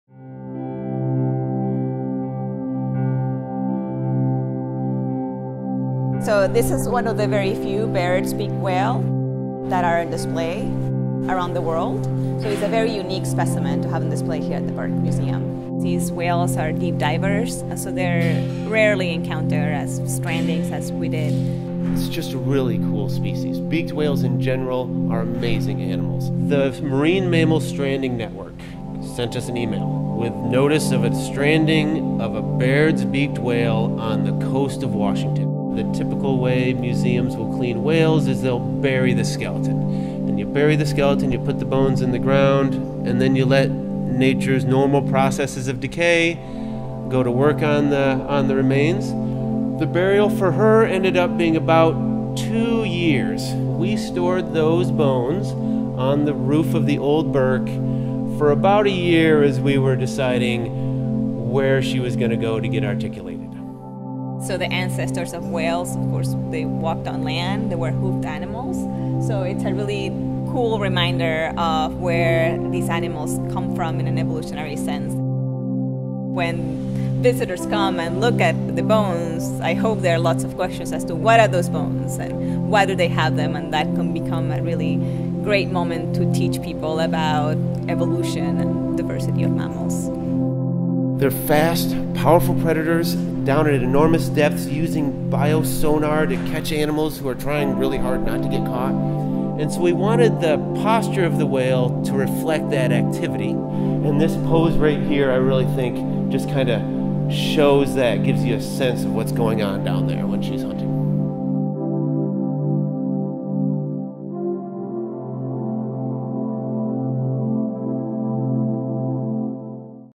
Baird's Beaked Whale at the Burke Museum
The 39-foot-long Baird's beaked whale (Beradius bairdii) displayed in the Burke Museum lobby washed ashore on a Washington beach in 2015. Preparing this specimen for display took more than two years, but it was worth it! This skeleton is now one of about 10 Baird's beaked whales in U.S. museum collections and offers incredible insights into a species that is rarely seen or studied.